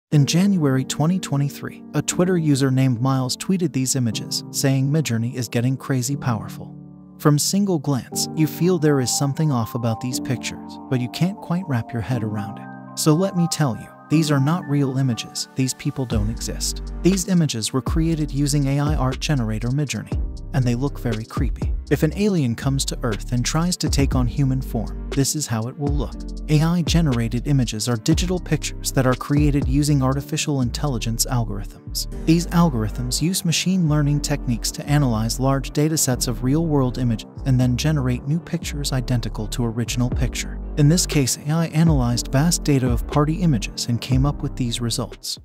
These People Don't Exist
Midjourney is an artificial intelligence program created by a San Francisco-based independent research lab Midjourney, Inc.
Midjourney generates images from natural language descriptions, called "prompts", similar to OpenAI's DALL-E and Stable Diffusion.
The tool is currently in open beta, which it entered on July 12, 2022.
The Midjourney team is led by David Holz, who co-founded Leap Motion.
Holz told The Register in August 2022 that the company was already profitable.
Users create artwork with Midjourney using Discord bot commands.